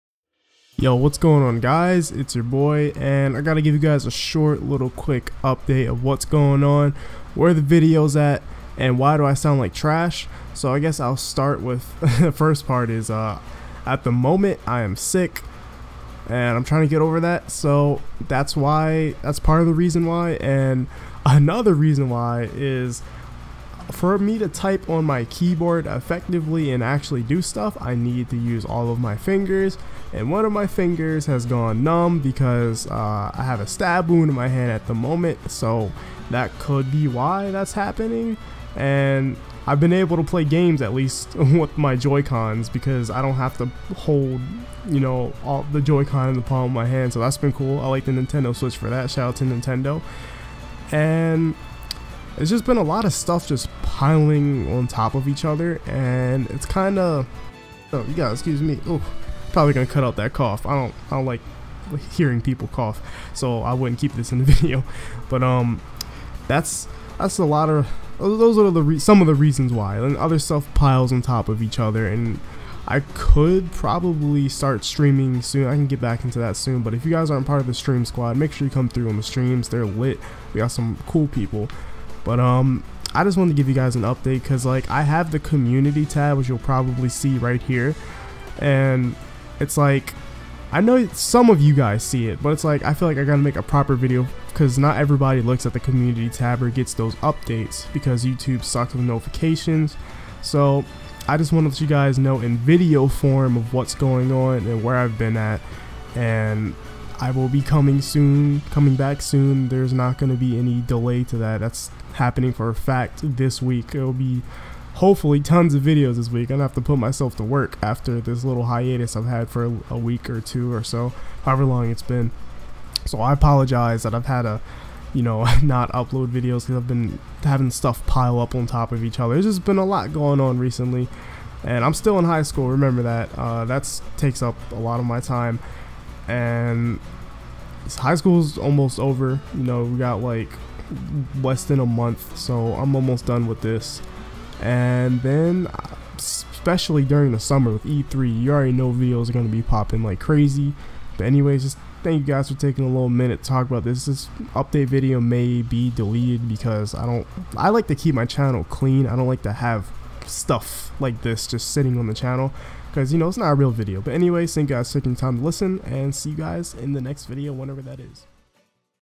What’s going on with ME and When videos will return - Important update
Thank you for taking the time to watch this update video it really shows the support and love for the channel and I cant keep going without you guys
▬▬▬▬▬▬▬▬▬▬▬▬ஜ۩۞۩ஜ▬▬▬▬▬▬▬▬▬▬▬▬▬
SQUAD (Not everyone):
Kingdomace:

uWiD8X5RJLwQcAbg